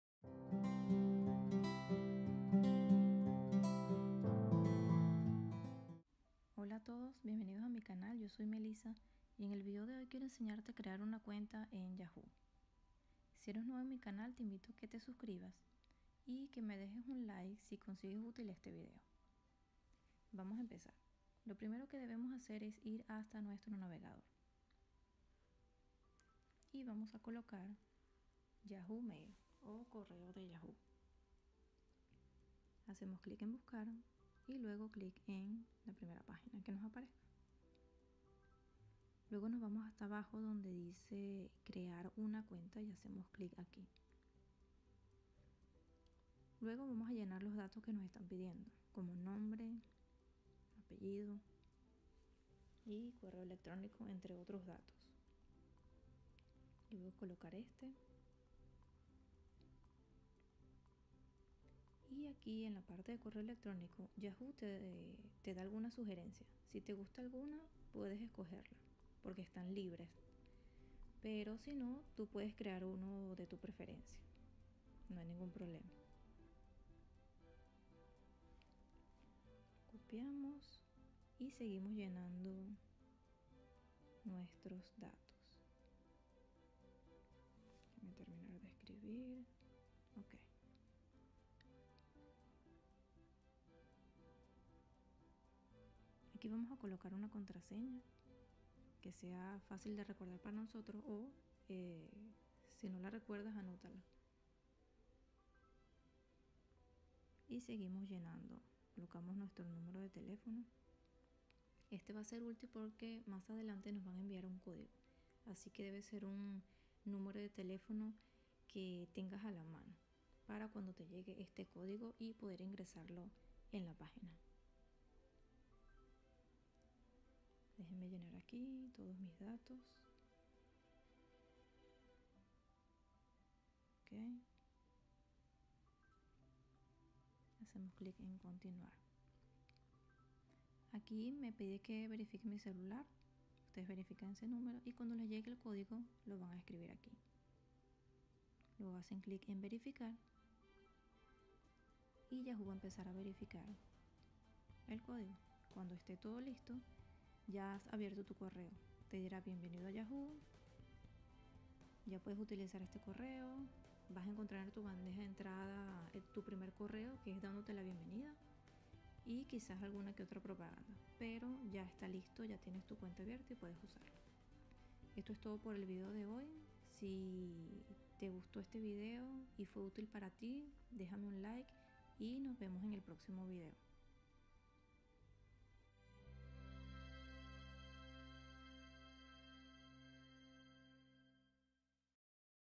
Cómo Crear Una Cuenta En Yahoo Mail 2021 | Melissa Allvarez
¡Bienvenidos nuevamente a mi canal! En este video te enseño a abrir una cuenta de correo en Yahoo.

Espero que sea de gran ayuda para ustedes!

Melissa Allvarez 🤍